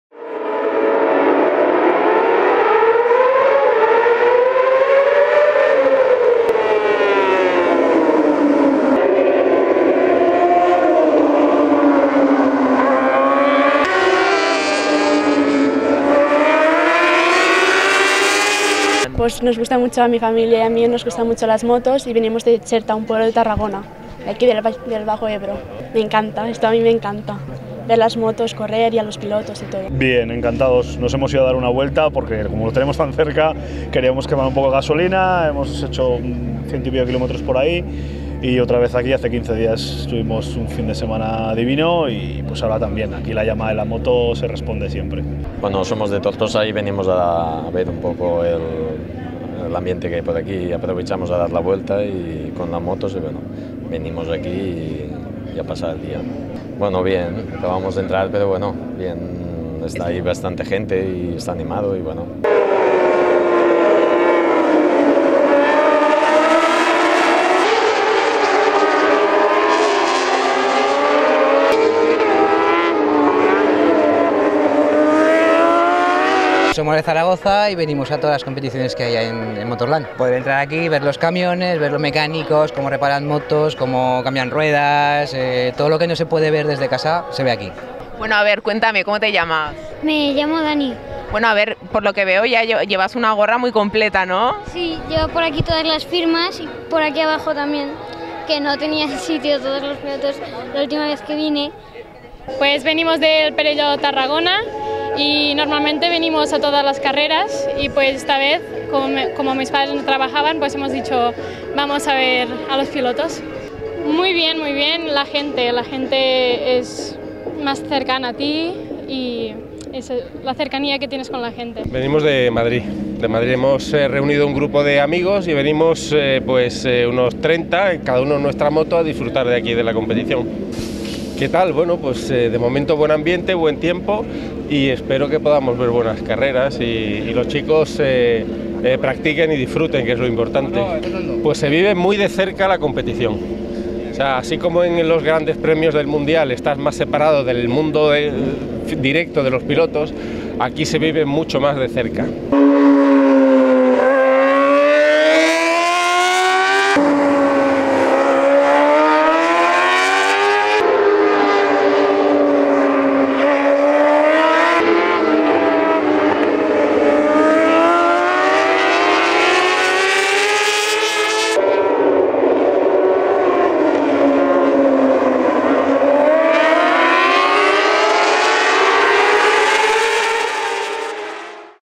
La Comarca.tv - FIM CEV REPSOL MOTORLAND 2017
Durante el fin de semana del 7 y 8 de octubre, Motorland Aragón ha celebrado el Fim Cev Repsol de motociclismo donde numeros pilotos han participado con la mirada puesta siempre en el mundial de motoGP. Buen tiempo en las instalaciones del complejo del motor y público llegado de difrentes partes de la geografía española. Gran ambiente y buenas carreras las vividas en la mañana del domingo 8 de octubre.